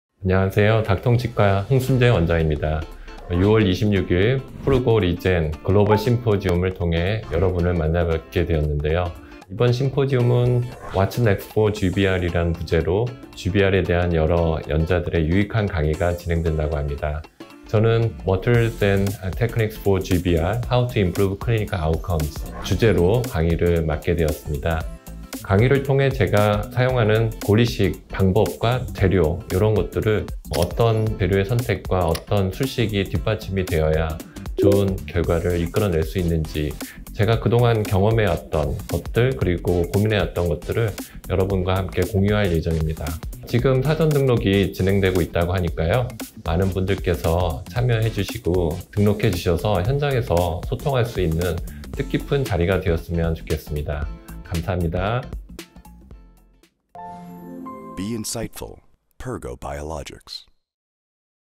2022 Purgo Regen Global Symposium Interview [Dr. Soon Xae Hong]
2022 Purgo Regen Global Symposium Interview [Dr. Soon Xae Hong]

주제 : Whtat's Next for GBR?
날짜 : 2022년 6월 26일 (일)
시간 : AM 09:30 ~ PM 16:00
장소 : Grand InterContinental Seoul Parnas Orchid

*2022년 6월 26일 PurgoRegen Global Symposium (in Seoul)이 열릴 예정입니다.